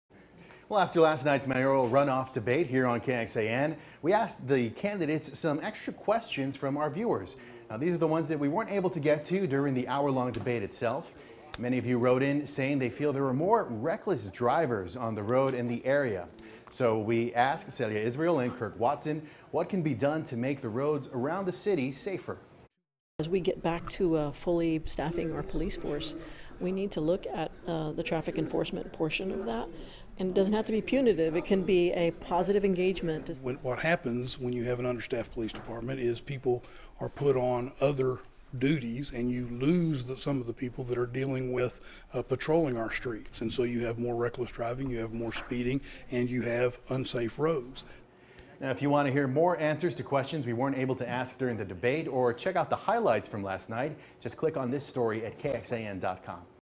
Deciding between the Austin mayoral candidates? Check out their stances on some big issues
KXAN asked our viewers what questions they had for Austin’s future mayor. We received hundreds of well-articulated submissions.